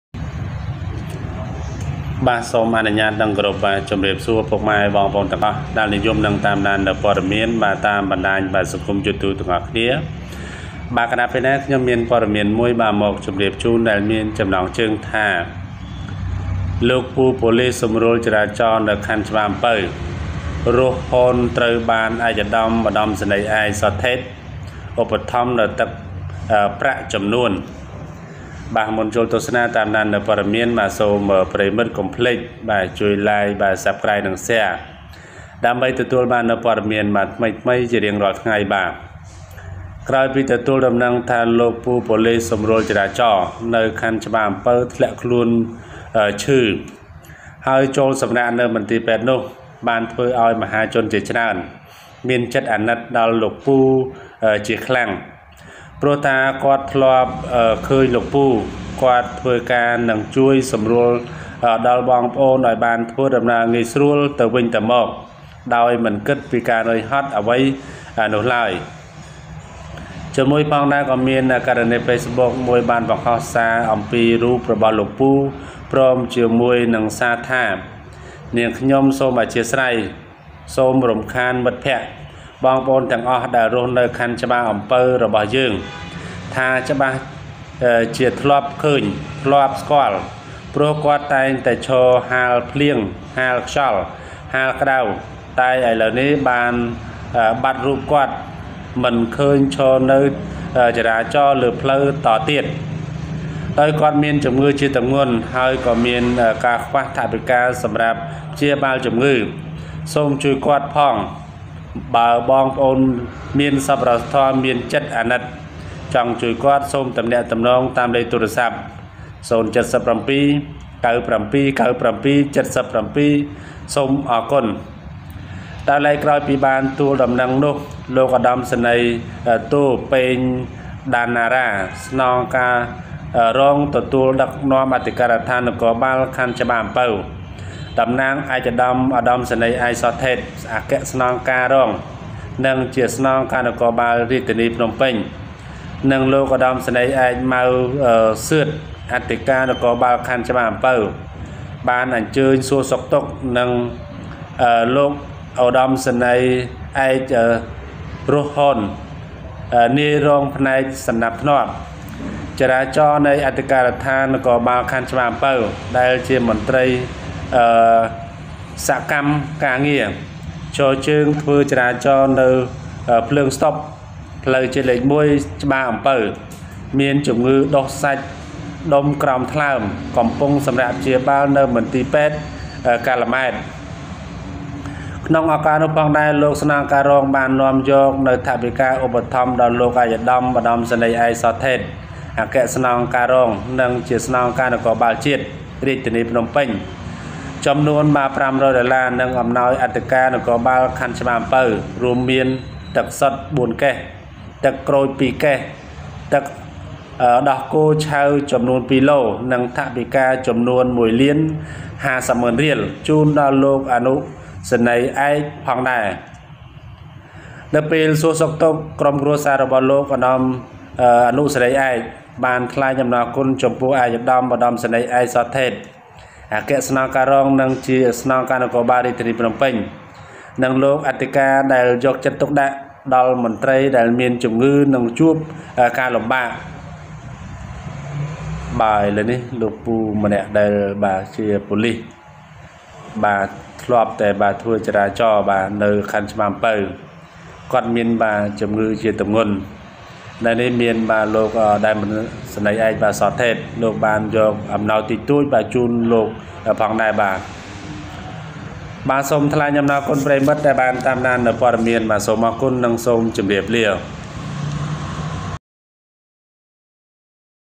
លោកពូប៉ូលីសសម្រួលចរាចរណ៍ នៅខណ្ឌច្បារអំពៅ រស់ ហ៊ន ត្រូវបាន ឯកឧត្តម ឧត្តមសេនីយ៍ឯក ស ថេត ឧបត្ថម្ភ នូវទឹ
The traffic police chief in Chbar Ampov district, Ros Horn, was supported by His Excellency Lt. Gen. Thet ...